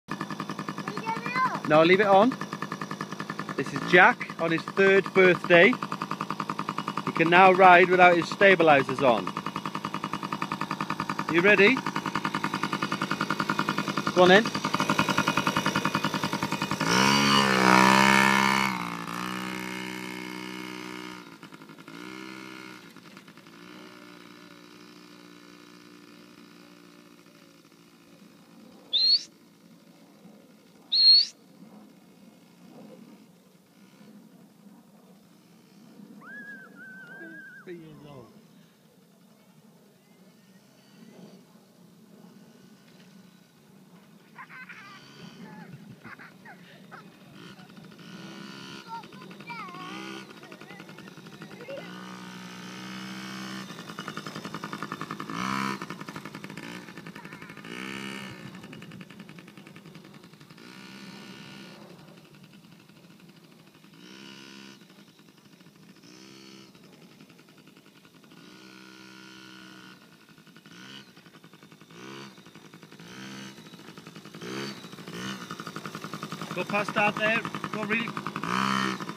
Jack styles pw50 age 3
Jacks first time without stabilises on his Yamaha pw50 age 3